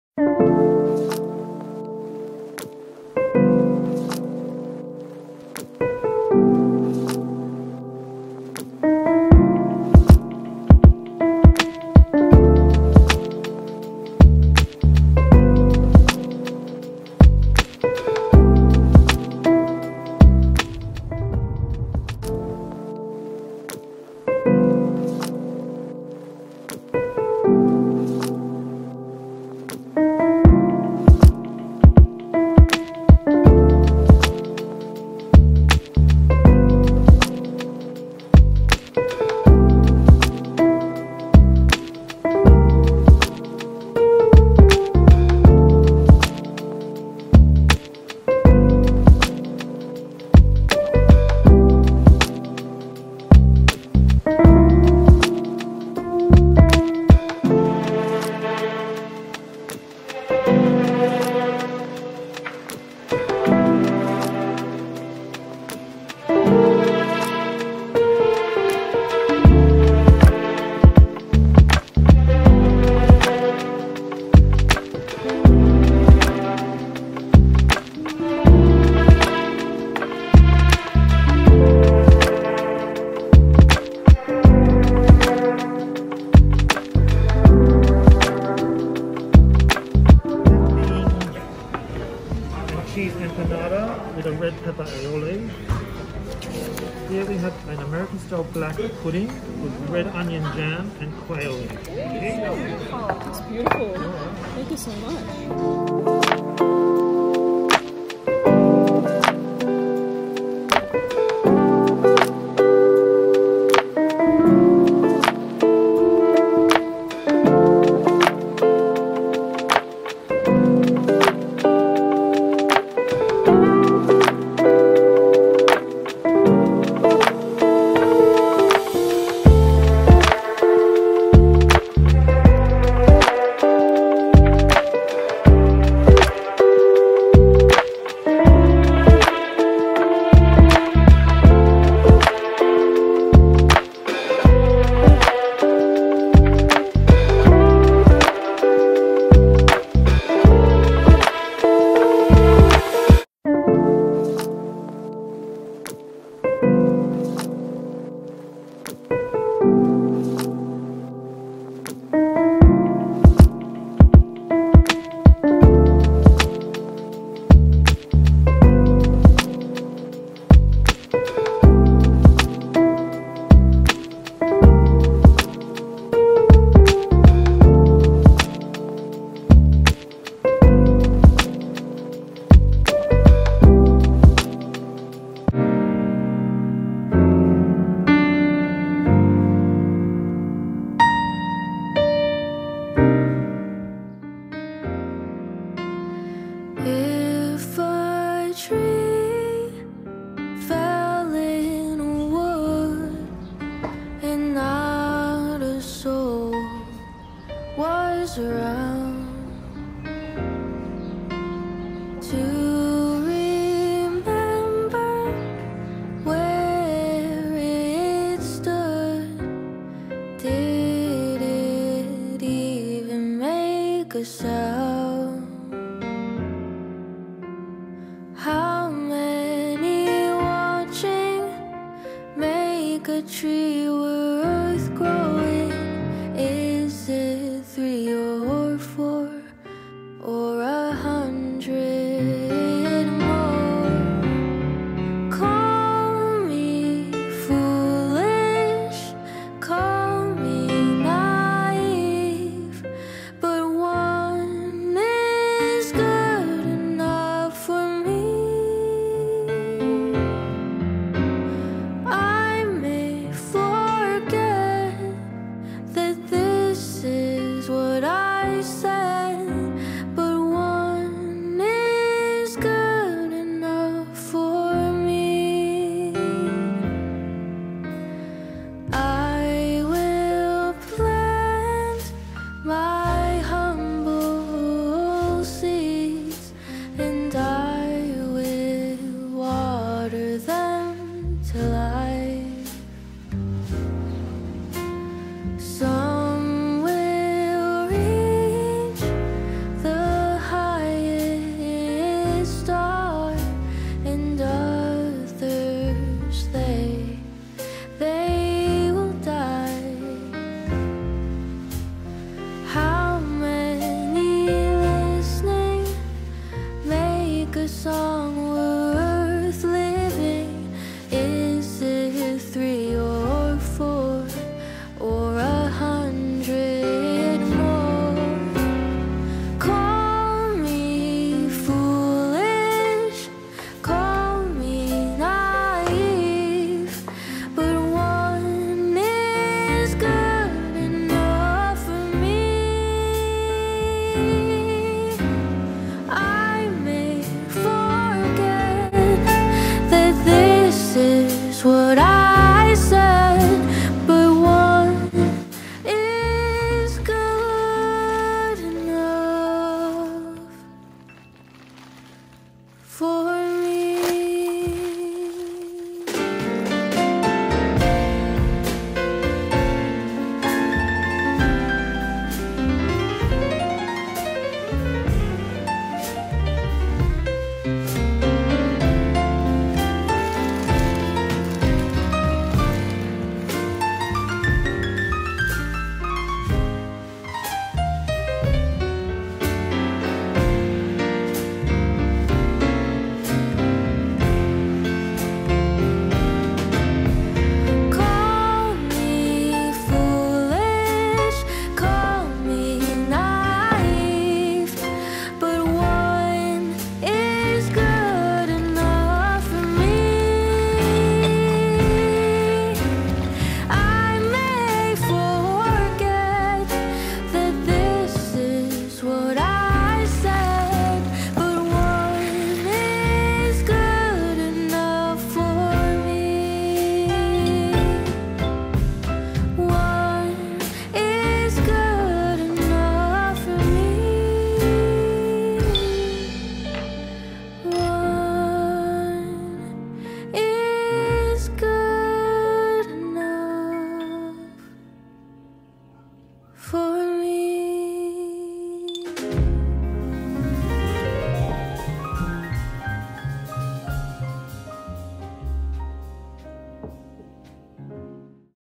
Melbourne Vlog ｜winery explore ｜ Antique Shop Treasure Hunt｜酒庄之旅｜墨尔本小镇古玩店寻宝
Delicious lunch at wine restaurant | Walking at a small country town | Browsing at antique & gift shop